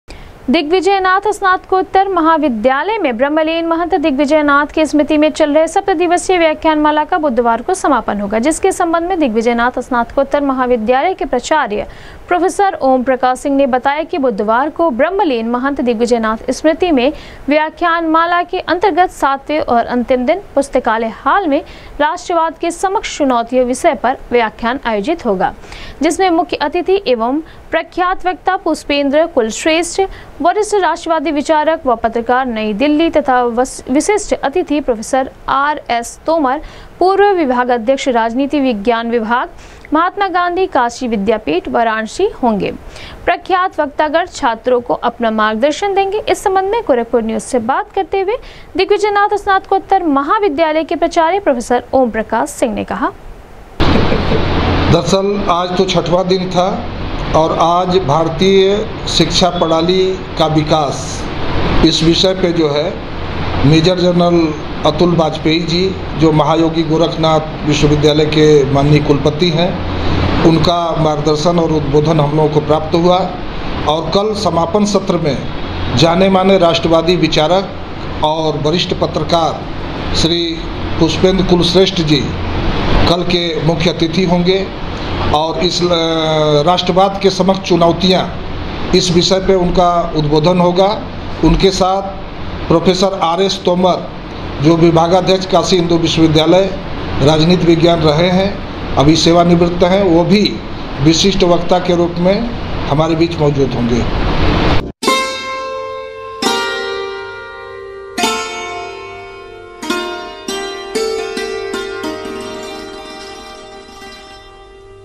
ब्रम्हलीन महन्त दिग्विजयनाथ के स्मृति में चल रहे सप्त दिवसीय व्याख्यानमाला का बुधवार को होगा समापन,
ब्रम्हलीन महन्त दिग्विजयनाथ के स्मृति में चल रहे सप्त दिवसीय व्याख्यानमाला का बुधवार को होगा समापन, मुख्य अतिथि के रूप में प्रख्यात वक्ता पुष्पेंद्र कुलश्रेष्ठ होंगे शामिल, विद्यार्थियों को देंगे अपना मार्गदर्शन।।